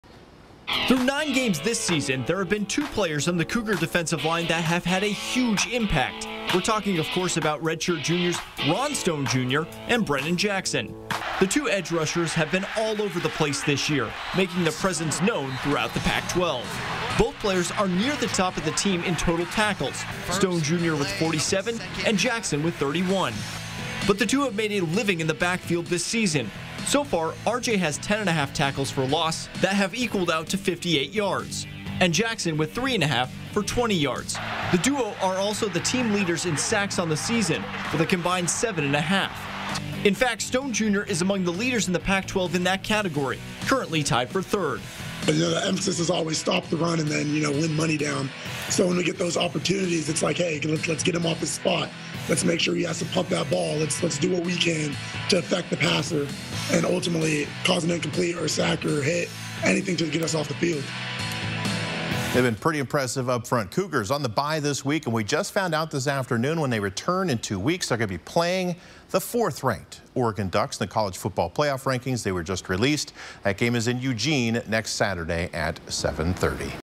Chalk Talk: Cougar edge rushers proving to be dominant in the Pac-12
As good as the Washington State offense has started to look in the middle part of the season, it's the defense that has really emerged as a force, causing mayhem and frustration for opposing offenses.

That starts at the line and getting pressure in early.  Through nine games this season, there have been two players on the cougar defensive line that have had a huge impact.  We're talking of course about red-shirt juniors, Ron stone Jr. and Brennan Jackson.

The two edge rushers have been all over the place this year, making their presence known throughout the Pac-12.  Both players are near the top of the team in total tackles: Stone Jr. with 47 and Jackson with 31.

However, the two have made a living in the backfields this season.  So far, Stone Jr. has 10.5 tackles for loss that have equaled out to 58 yards, and Jackson with 3.5 for 20 yards.  The duo are also the team leaders in sacks on the season, with a combined 7.5, and Stone Jr. is tied for third in the conference for sacks.

"The emphasis is always: stop the run and then win money down. So when we get those opportunities, it's like hey let's get him off his spot let's make sure he has to pump that ball. Let's do what we can to affect the passer and ultimately cause an incompletion or sack or hit, anything to get us off the field," said Stone Jr.

The Cougars will now head into their bye week, before heading down to Eugene to play the Oregon Ducks on Nov. 13.